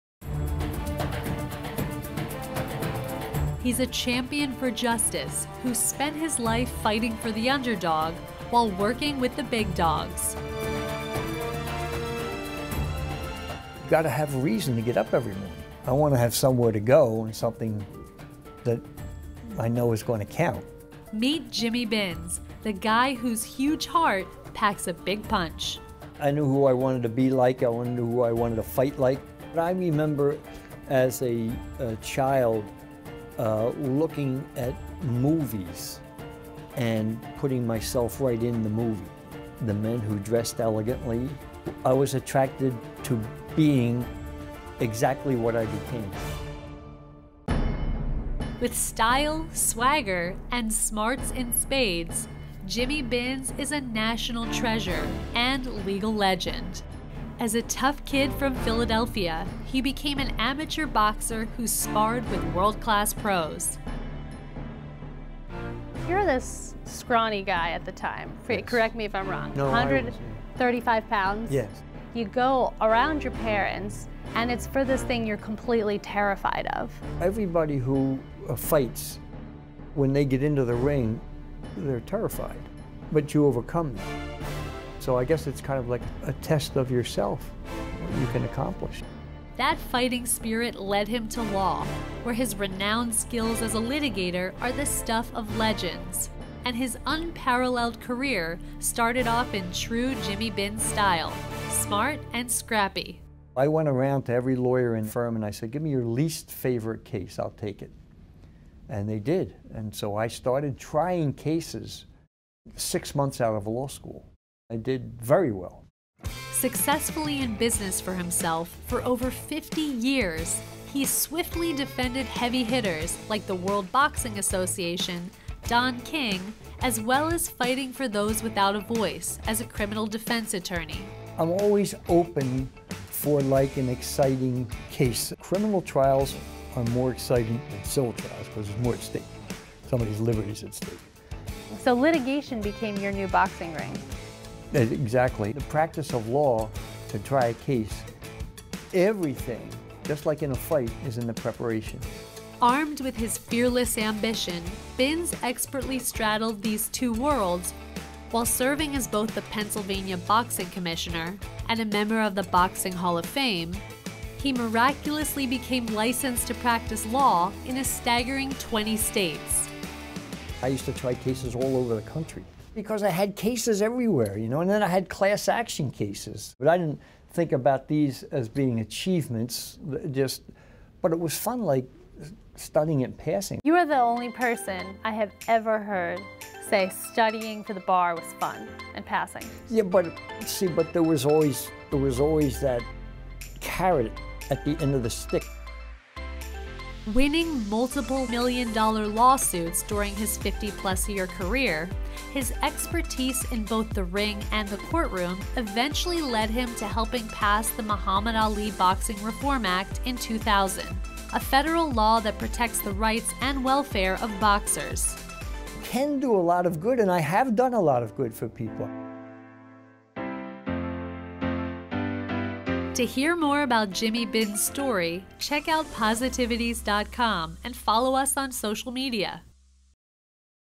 Meet Jimmy Binns - The Hero Among Us
Jimmy Binns is the definition of a jack of all trades. At 74 years old, he started a new career - working as a police officer. Throughout his 74 years he's been a philanthropist, lawyer, police offer and much more. Most notably, he represented Don King against Mike Tyson. His determination to continue excelling is a true inspiration.